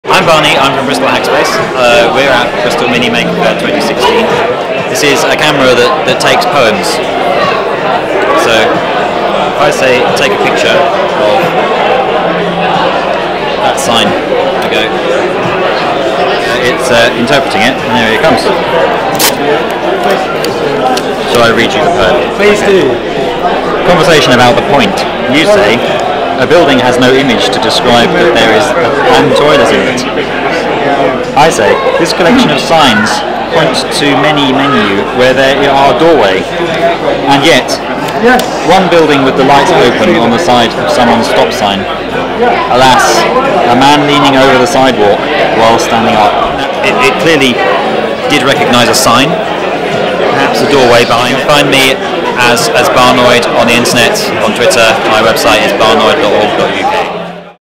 Poetry Polaroid by Barney "Barnoid" Livingston Bristol Mini Maker Faire 2016
Barney aka @barnoid of barnoid.org and Bristol Hackspace shows us his Polaroid camera converted to make poetry from what it sees. Using an Adafruit thermal printer, Raspberry Pi with camera and other hardware and clever programming, the camera interprets what it sees into several forms of poetry.